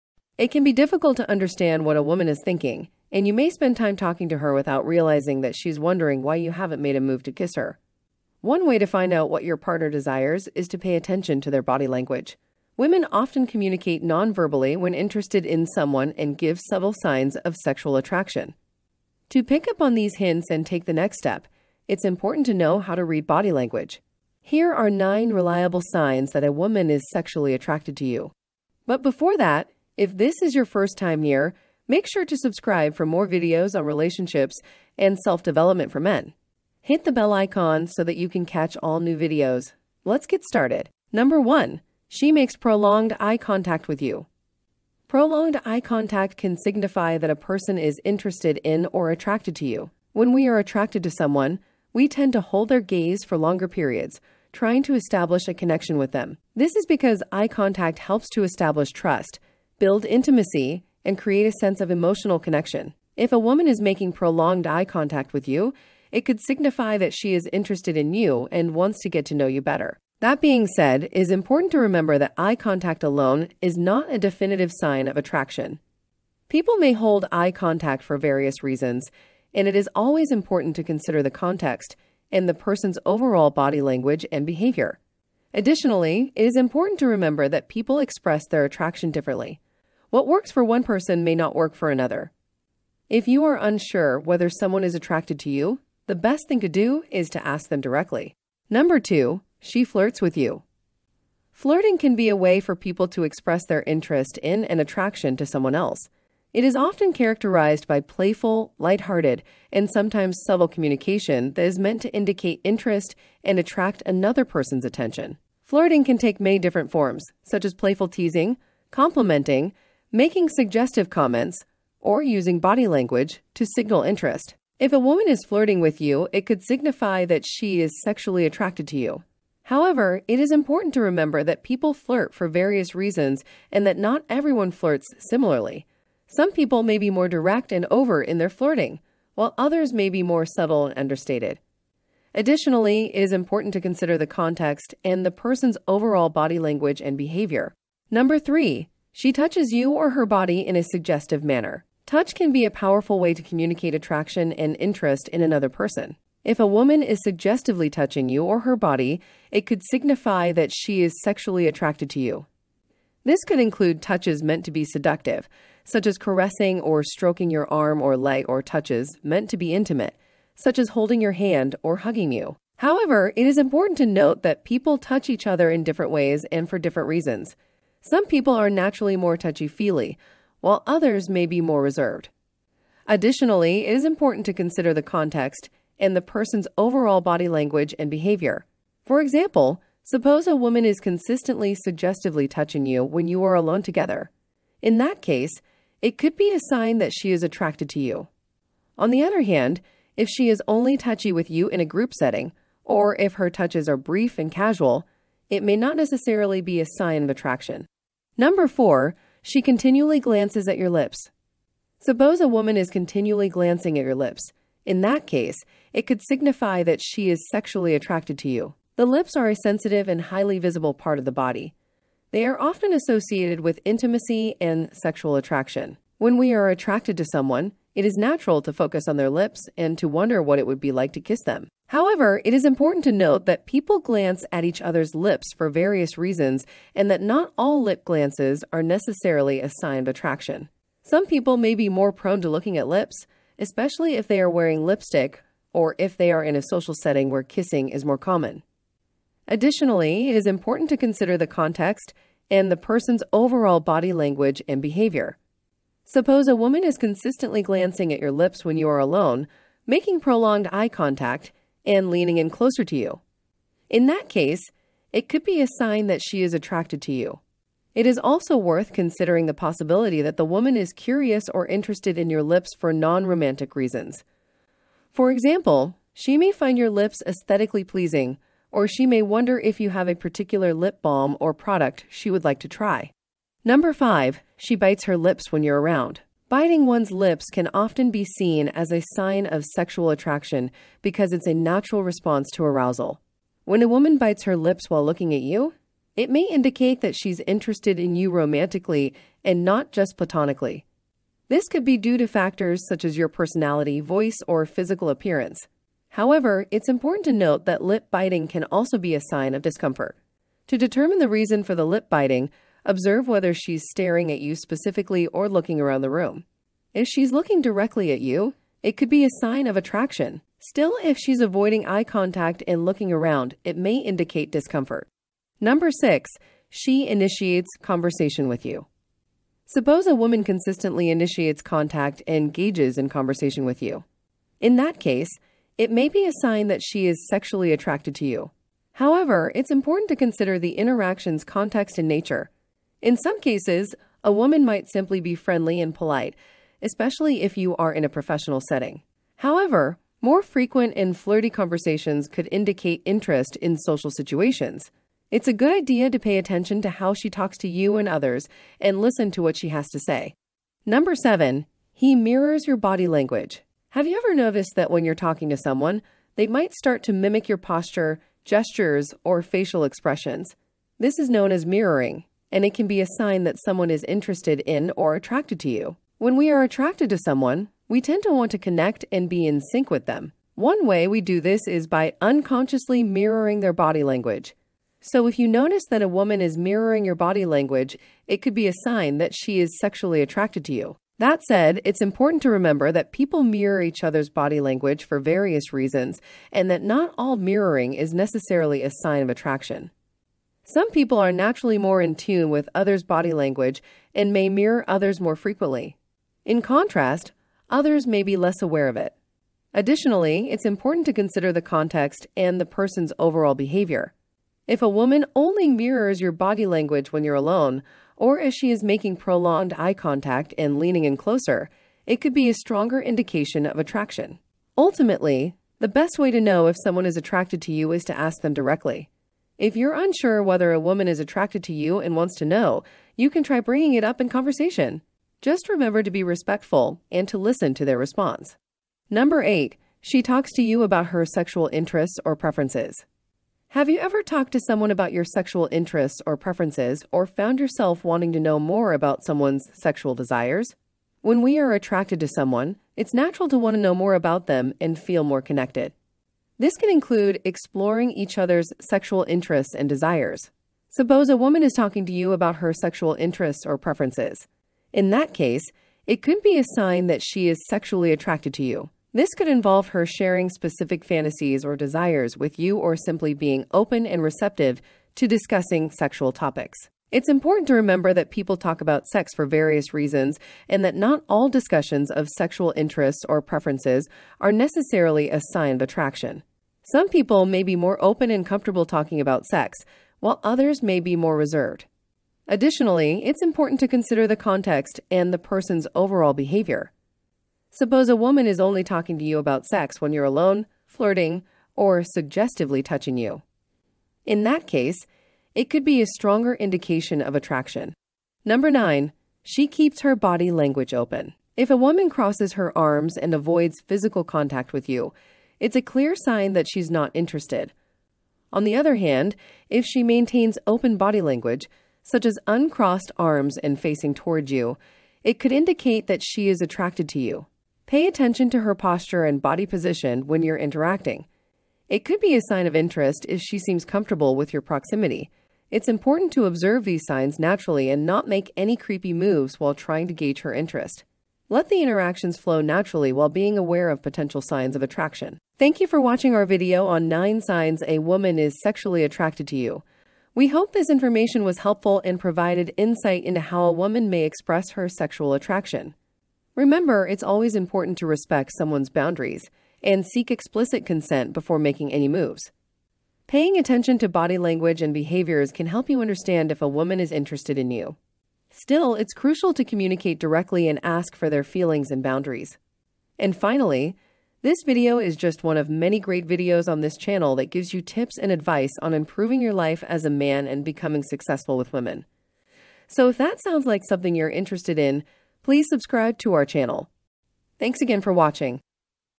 09 Signs A Women is Sexually Attracted to You
It can be difficult to understand what a woman is thinking, and you may spend time talking to her without realizing that she's wondering why you haven't made a move to kiss her. One way to find out what your partner desires is to pay attention to their body language. Women often communicate non-verbally when interested in someone and give subtle signs of sexual attraction. To pick up on these hints and take the next step, it's important to know how to read body language. Here are 09 reliable signs that a woman is sexually attracted to you.

Couple kisses image designed by HAKAKATb at VectorStock.com